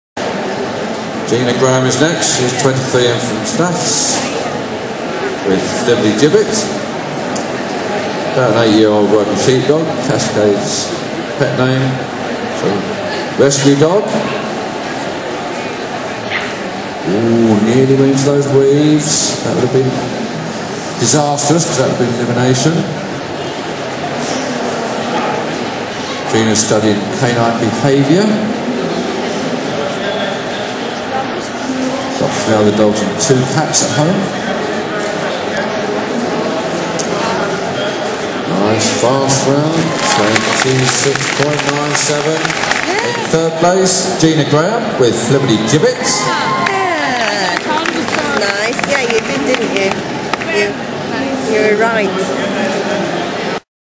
Crufts 2008 - YKC Senior Jumping - 3rd
Cas (Flibberty Gibbet), almost getting E'd in the weaves, but going on to come 3rd. :-)